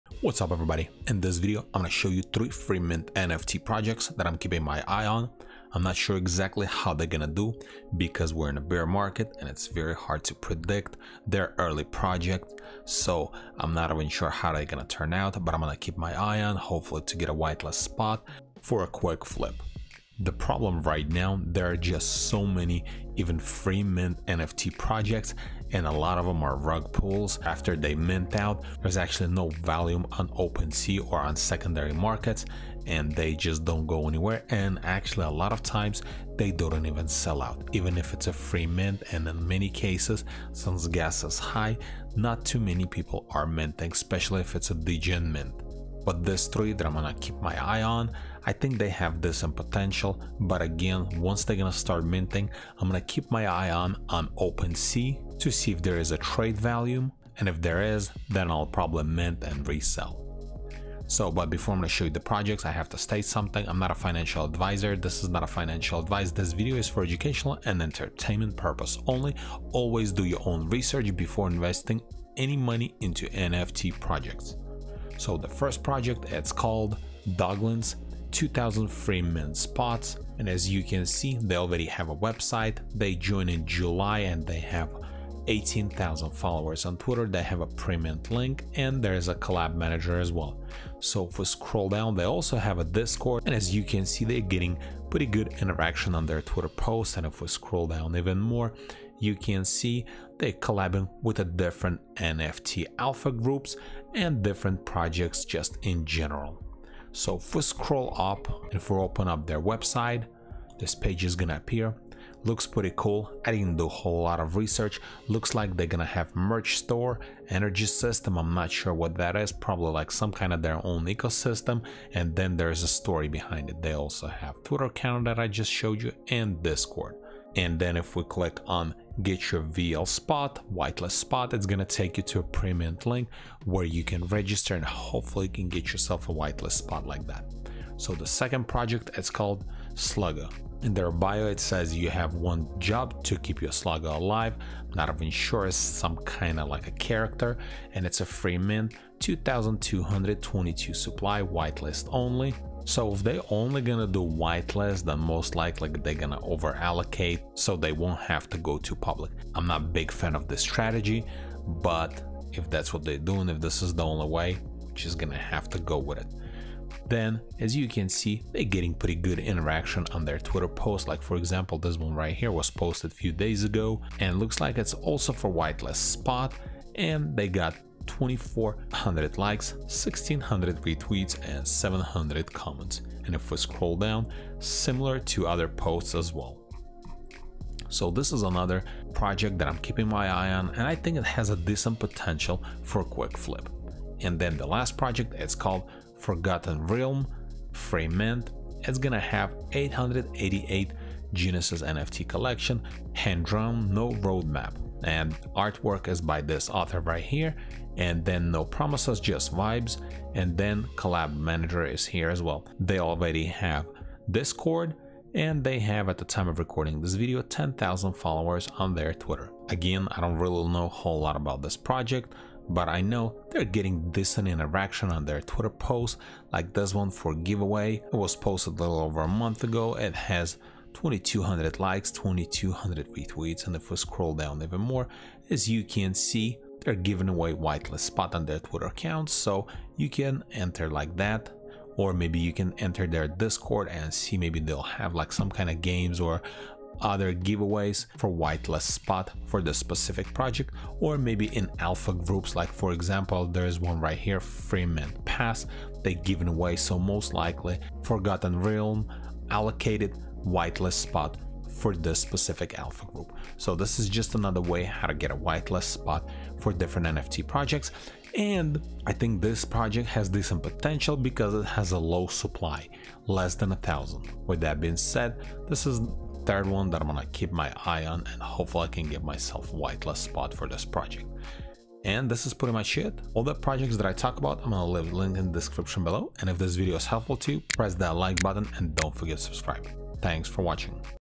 3 FREE Mint NFT Projects
In this video I will show you 3 projects with free mint NFTs

1. ➡ Doglins 👇
Mint Date: TBA

2. ➡ Slugga 👇
Mint Date: TBA

3. ➡ FORGOTTEN REALMS  👇
Mint Date: TBA

PERSONAL GEAR
4k Asus Monitor
Video Camera Tripod